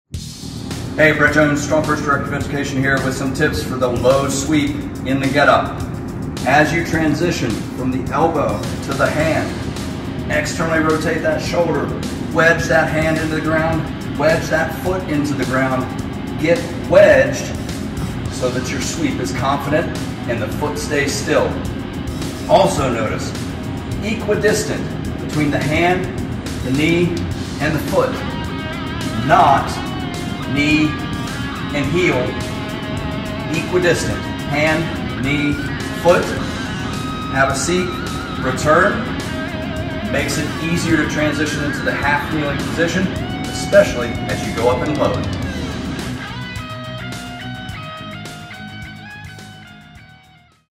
Kettlebell Get-Up: Correct Low Sweep Alignment | StrongFirst
Pay attention to your sweep. The low sweep should end up with the foot of the up leg, the knee of the leg that was swept back, and the hand on the ground equidistant from each other. This alignment means a subtle hinge to keep the kettlebell over your center of mass, resulting in a solid transition to half-kneeling. When you over-sweep the leg so the knee is closer to the front foot, it places you in an overextended position where you either cannot handle the transition or the transition is so jerky it lacks control. Nail this step for a better get-up.

Also, during the sweep, you want to get wedged between the hand on the ground and the planted foot of the front leg. The foot does not shift or lift. This is one of the standards for those wanting to submit a Sinister attempt. Getting wedged between the heel of the hand and the heel of the foot changes the low sweep into a strong and confident move, not one where you are rushing, shifting, and losing control. Ignore it at your peril.

Check out the Kettlebell Simple & Sinister online course for more detailed instructions on the swing and get-up.